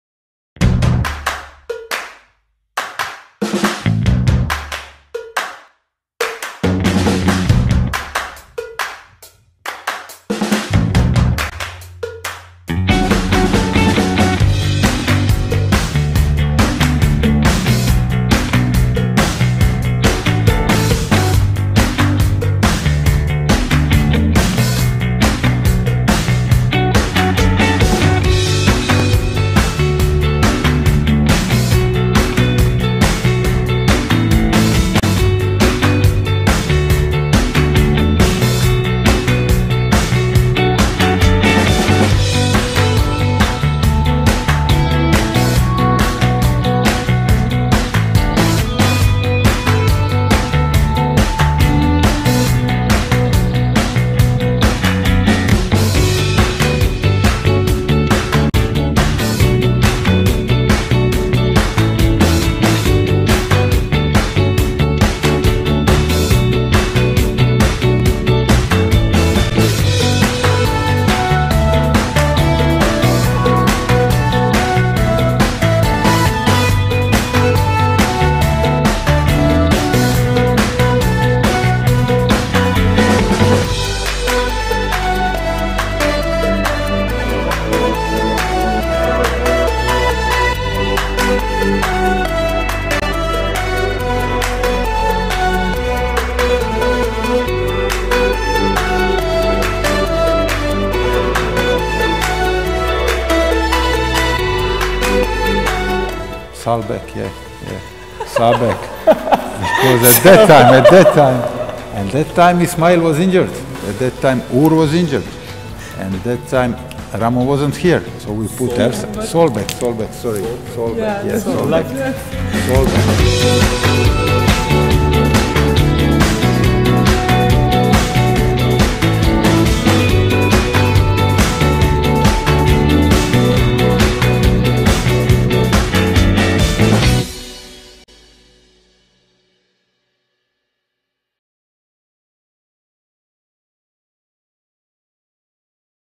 GQ Türkiye - Slaven Bilic kamera arkası
GQ Türkiye Aralık sayısı kapağı, futbolun asi kahramanı Slaven Bilic çekiminden özel kamera arkası görüntüleri...